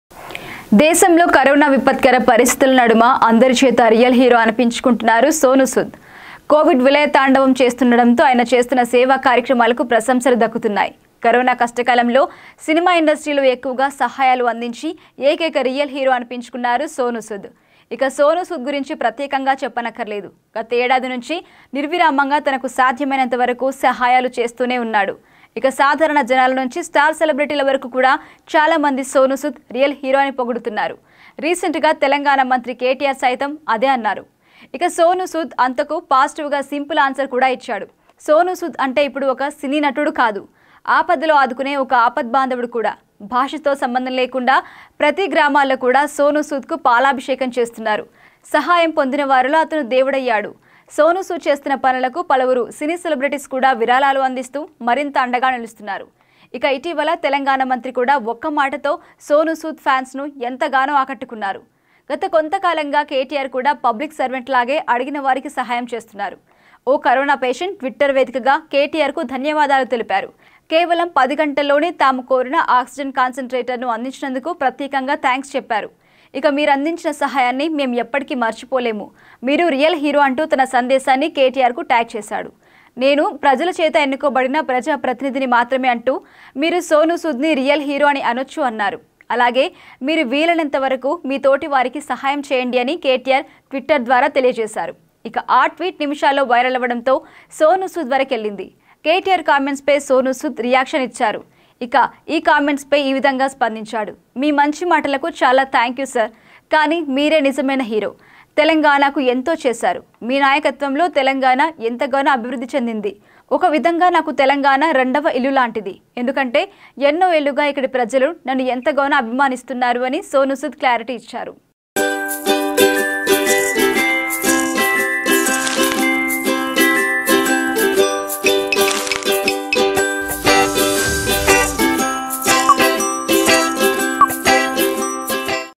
నేను కాదు, సోనూసూద్ సూపర్ హీరో..! | KTR Special Tweets on Sonu Sood a Real Hero | Sonu Sood Reaction
KTR Special Tweets on Sonu Sood a Real Hero | Sonu Sood React and Respond to KTR Tweets | Kavyas Media

included Topic :
New Telugu Movie Reviews,
Telugu News,
Latest Social Updates,
Biggboss Reviews&Analysis,
Social-Apps-Websites Updates,
Technical Supporter's,

Category
Entertainment